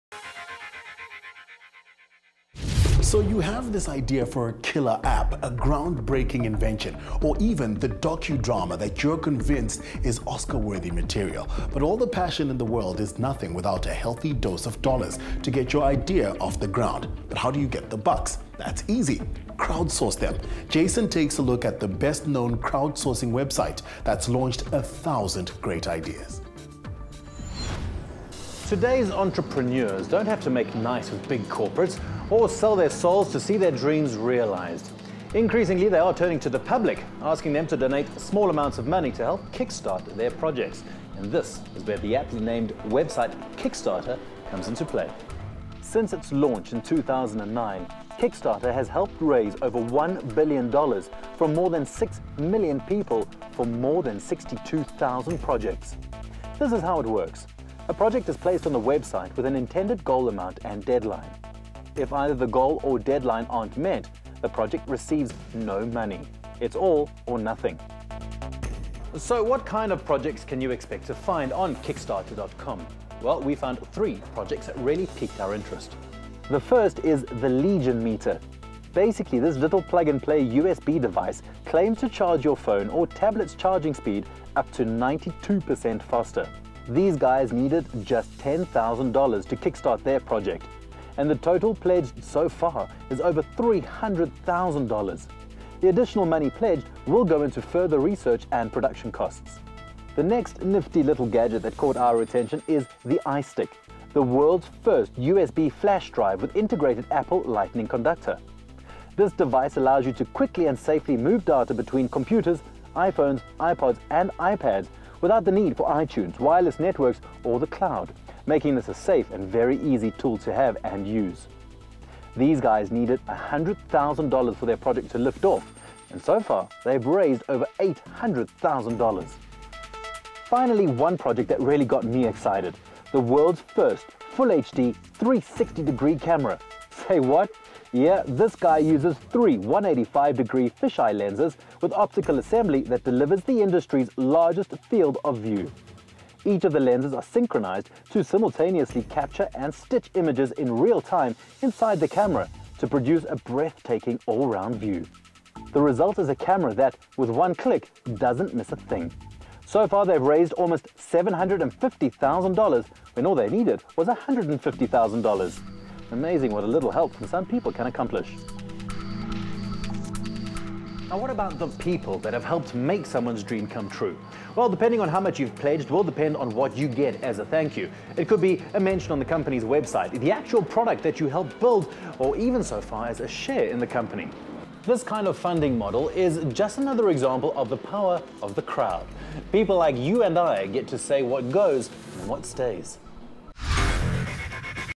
Tech Report | Kickstarter Review
So you have this idea for a killer app... a ground breaking invention... or even the docudrama that you're convinced is Oscar worthy material...
But all the passion in the world is nothing without a healthy dose of dollars to get your idea off the ground...
How do you get the bucks? That's easy -- crowd source them...
Jason takes a look at the best known crowd sourcing website that's launched a thousand great ideas.